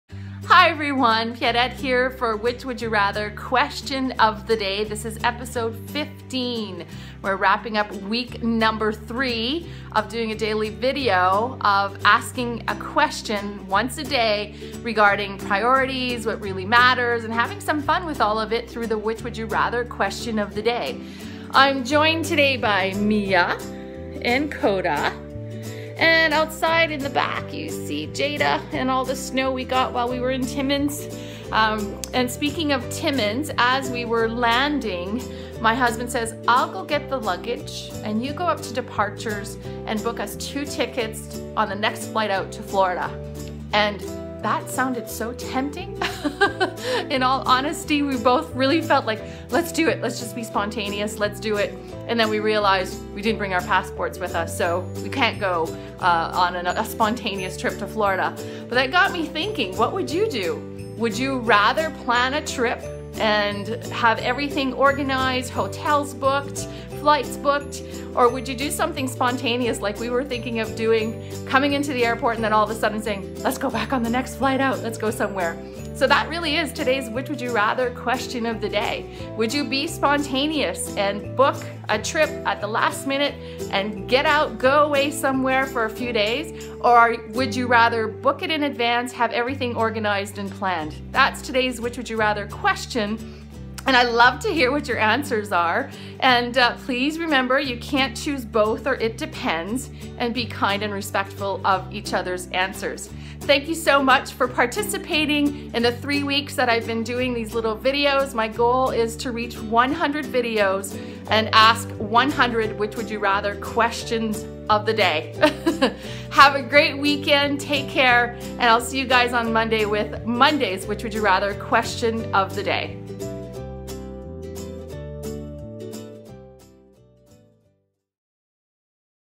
Which Would You Rather - Episode 015
Today's question is all about spontaneity vs having things planned. I would love to know your answer to today's question. Please post in the comments below and remember you can't choose "it depends" or "both", and be kind and respectful of each other's answer. So, which would you rather?